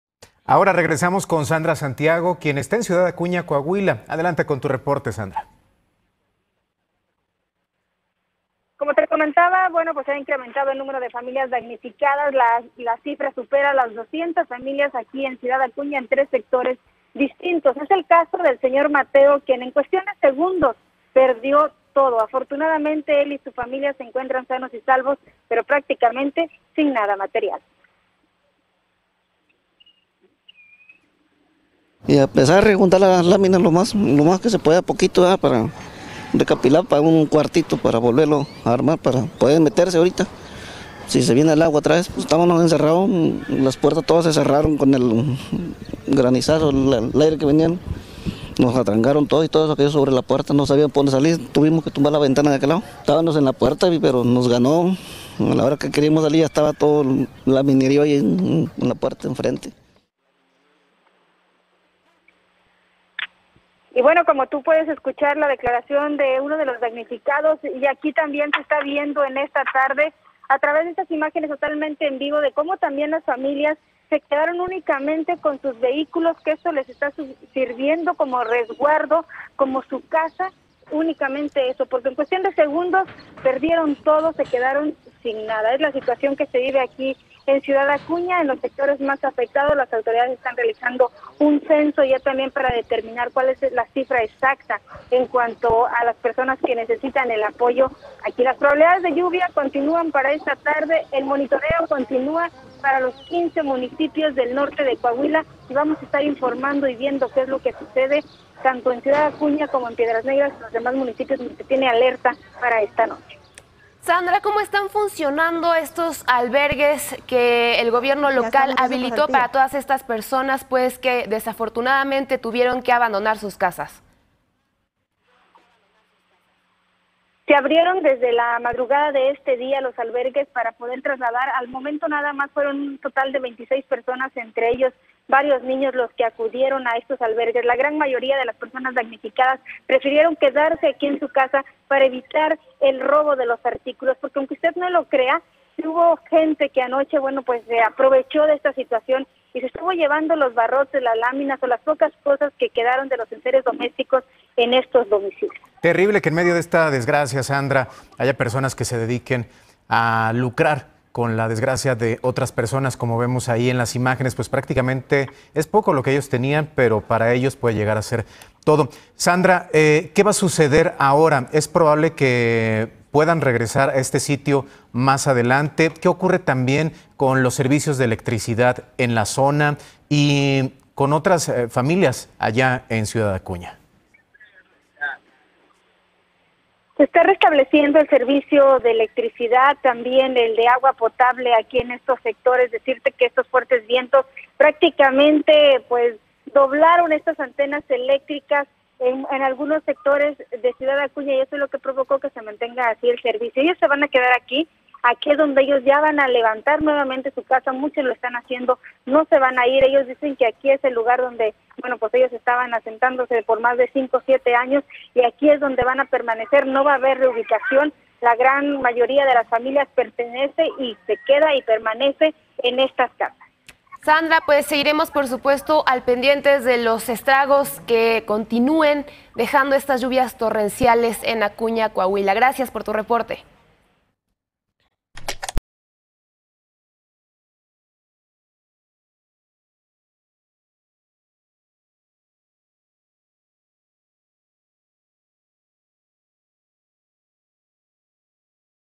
Lluvias en Ciudad Acuña dejan más de 200 familias damnificadas - N+ 15
En Ciudad Acuña, Coahuila, se registró una fuerte tormenta que causó estragos en distintas zonas de la comunidad, dejando al menos a 200 familias damnificadas. Los afectados, en su mayoría, vivían en casas con techo de lámina, por lo que se apresuran en construir nuevos cuartos para resguardarse en caso de que haya más lluvias.